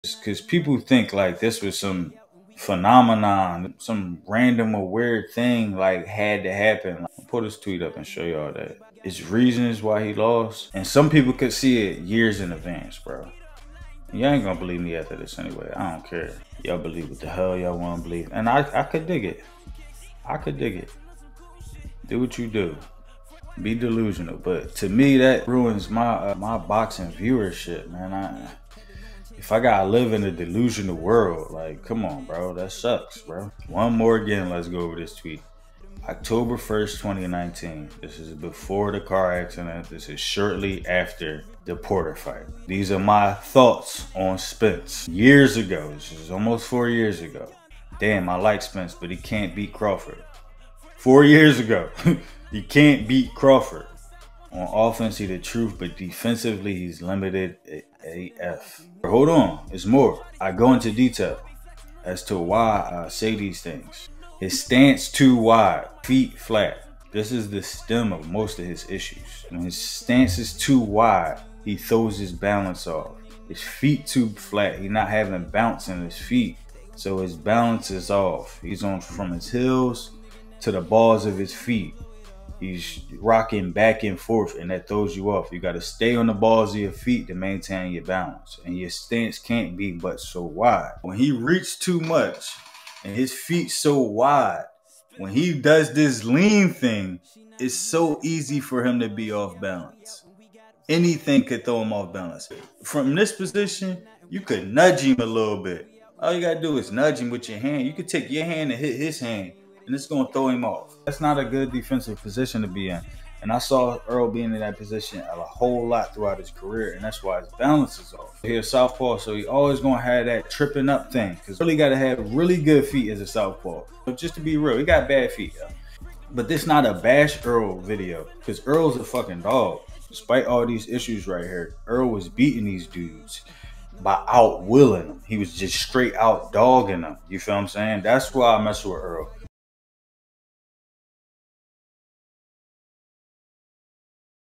Tweet from 2019 disproves all of the Errol Spence excuses and lies
This series of tweets from Oct 1, 2019 (before Errol's accident) disproves many of the excuses made after the Errol Spence Terrence Crawford showdown